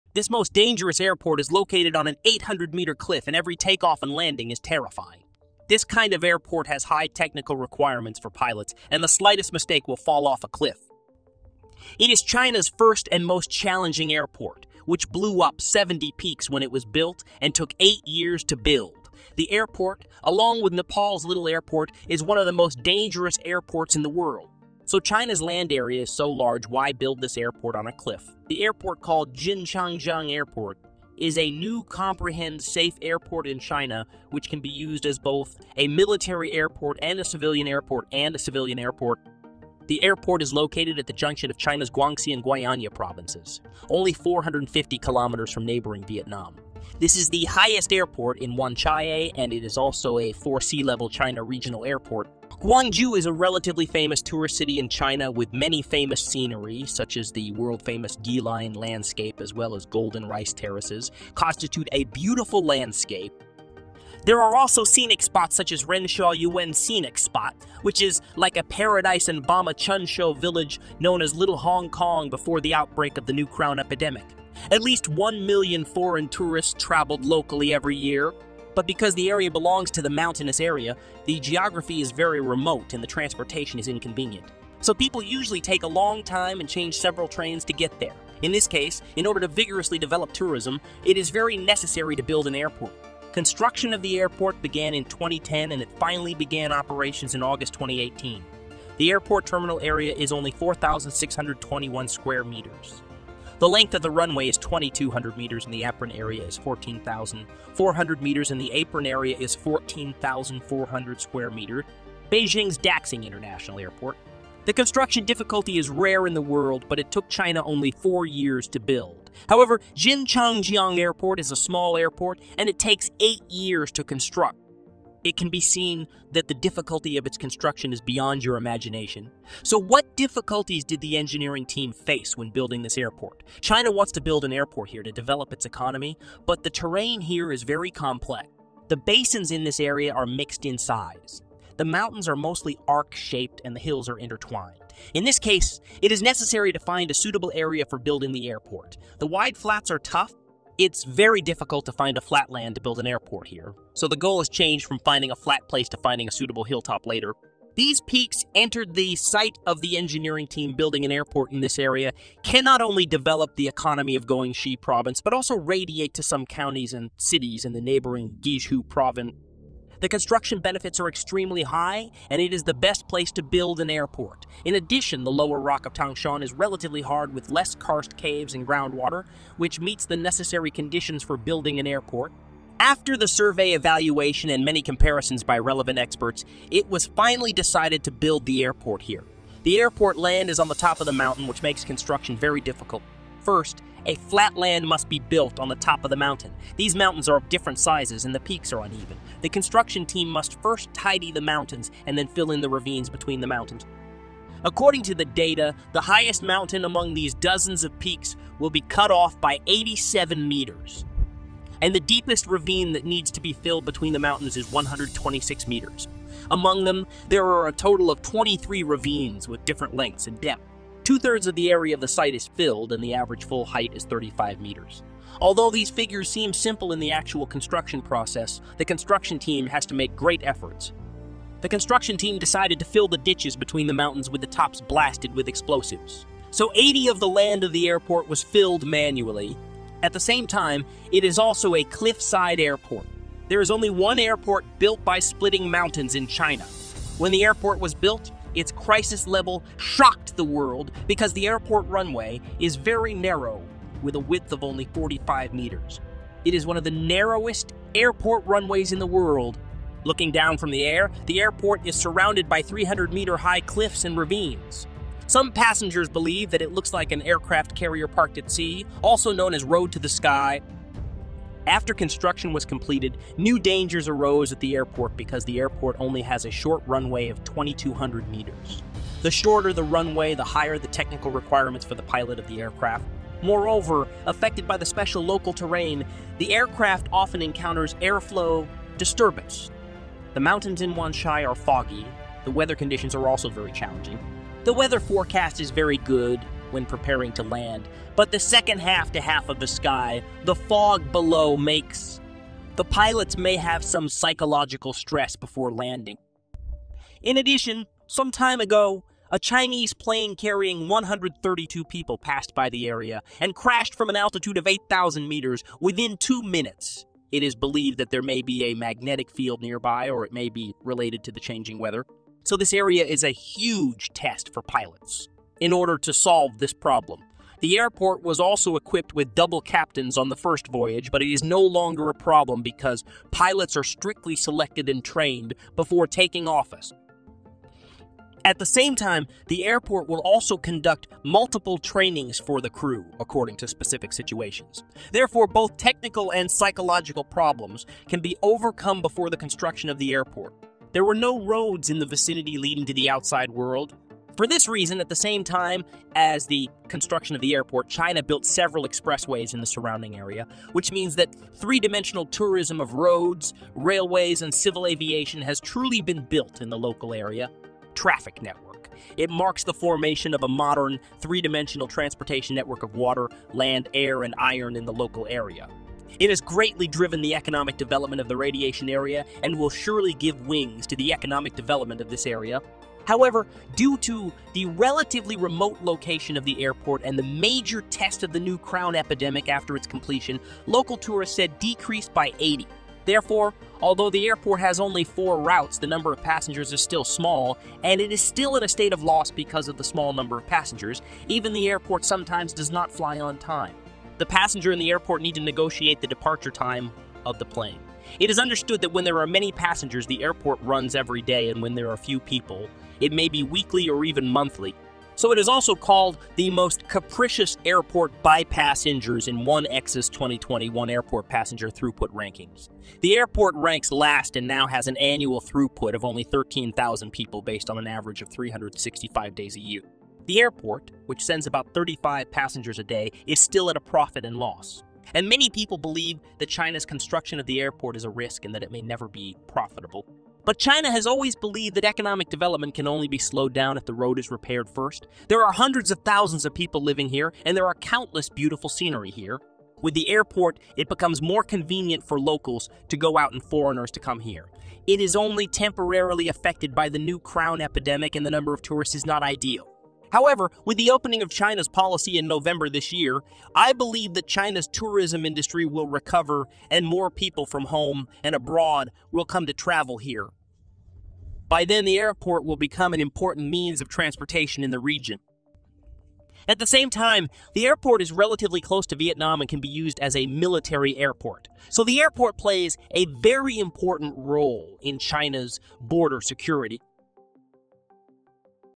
NEW China's INSANE Airport In Mountains! It Took China 8 YEARS Yo Build And 70 Mountains To Blow Up!
Every takeoff and landing at this most hazardous airport, which is perched on an 800-meter cliff, is horrifying. The technical requirements for pilots at this type of airport are enormous, and even the smallest error might result in disaster. It took eight years to build and blew out 70 peaks when it was completed, making it China's first and most difficult airport. The airport is among the most hazardous airports in the world, along with the little airport in Nepal.

Given the size of China's landmass, why was this airport constructed atop a cliff? Thanks for visiting the World Project. A brand-new, secure airport in China that can be utilized for both military and commercial purposes is named Jin Changjiang Airport. The airport is situated where the Chinese provinces of Guangxi and Guiania converge. just 450 miles away from Vietnam's border.

This airport is the tallest in Wanchai and a China Regional Airport for sea level. Golden rice terraces and other notable scenery make up a magnificent panorama in Gwangju, a reasonably well-known tourist destination in China. Other famous scenery includes the world-famous Guilin landscape. Additionally, there are picturesque areas like Renshaw Yuen Scenic Spot, which resembles a paradise, and Bama Chun display hamlet, once known as Little Hong Kong before the new Crown plague sprang out.

Every year, at least 1 million international visitors visit the area. However, the geography is incredibly secluded, and getting around is difficult because the area is hilly. To reach there, travelers typically travel slowly and change trains multiple times. Construction of an airport is very important in this situation in order to actively boost tourism.